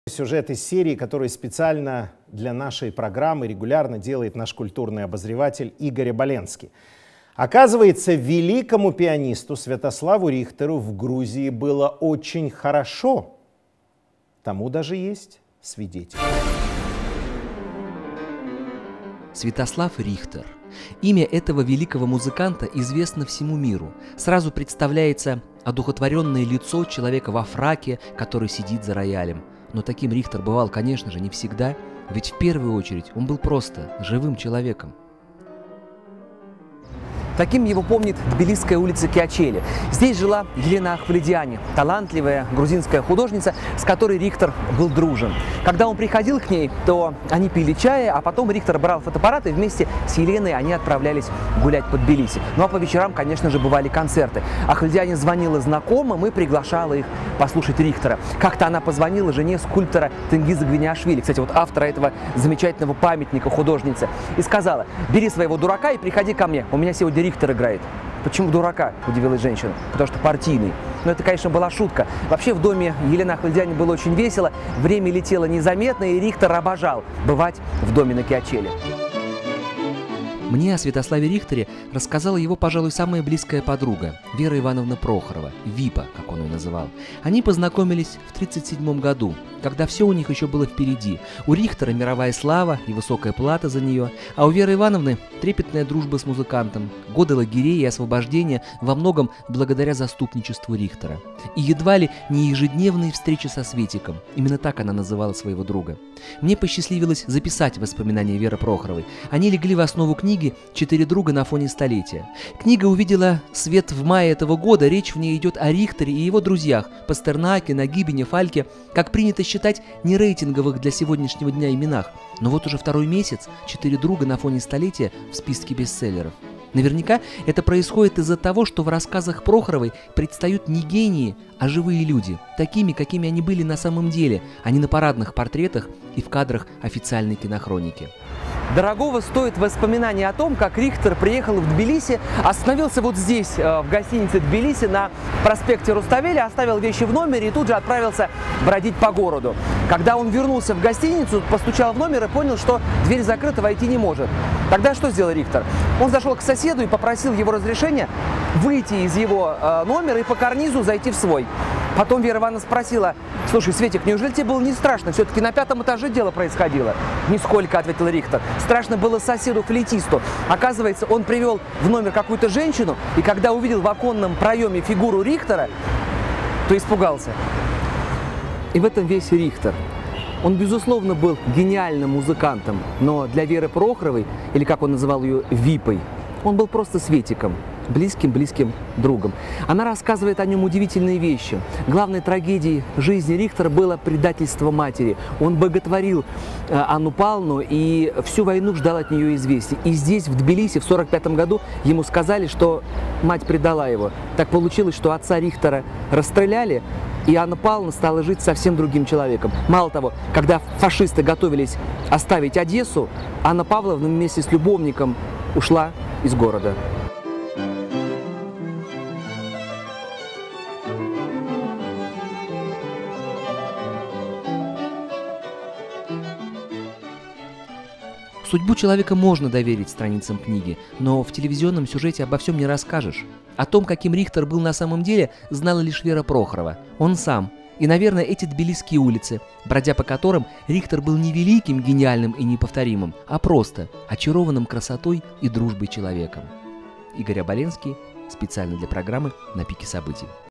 "НЕИЗВЕСТНЫЙ РИХТЕР"
История Святослава Рихтера, рассказанная в эфире программы Матвея Ганапольского.
Во время своих приездов в Тбилиси гениальный пианист раскрывался с неожиданной стороны...
Микрофильм Игоря Оболенского, в основу которого легла книга "Четыре друга на фоне столетия..."